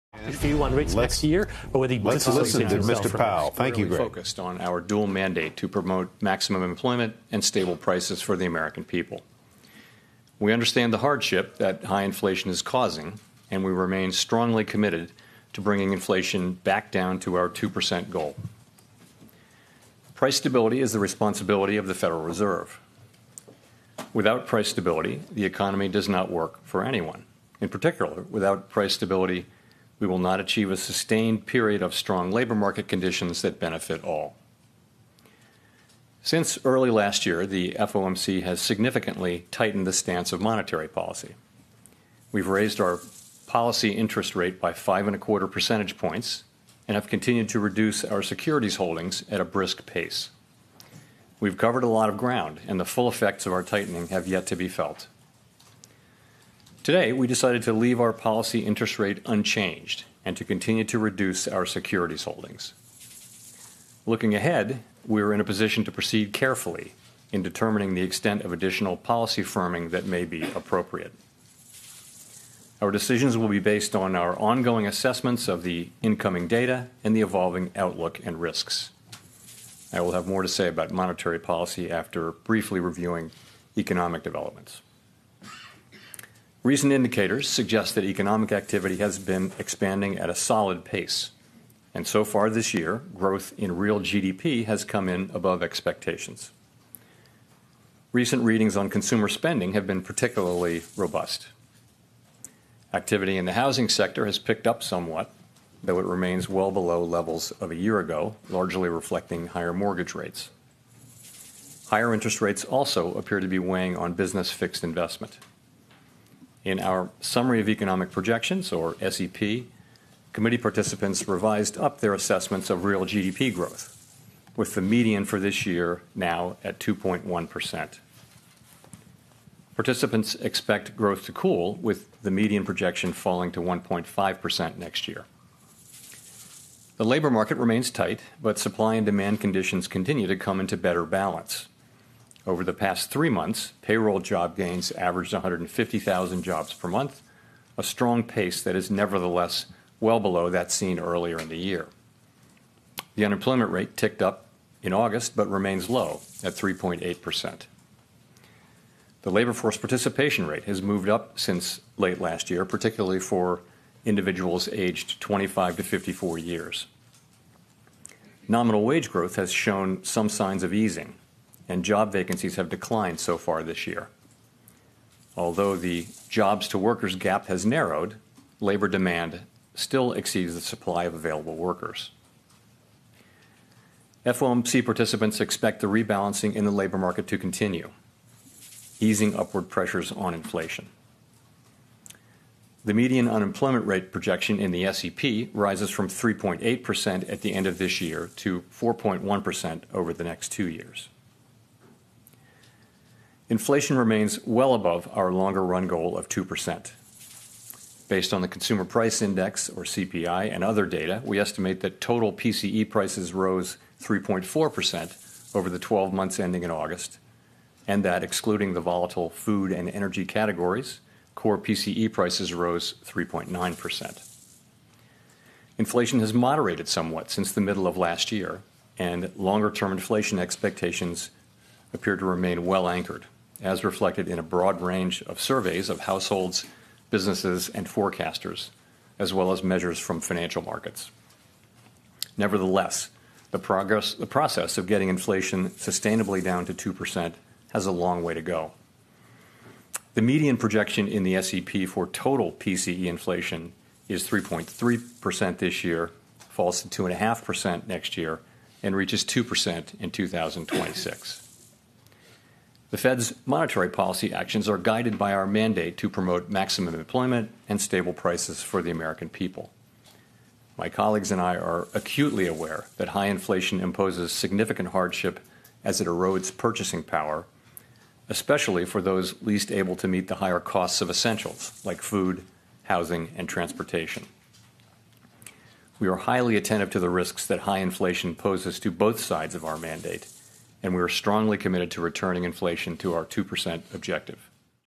Fed Chair Powell: We remain strongly committed to our 2% inflation goal
Federal Reserve Chair Jerome Powell talks the economy, inflation, unemployment and more after announcing the Fed would keep interest rates unchanged in September.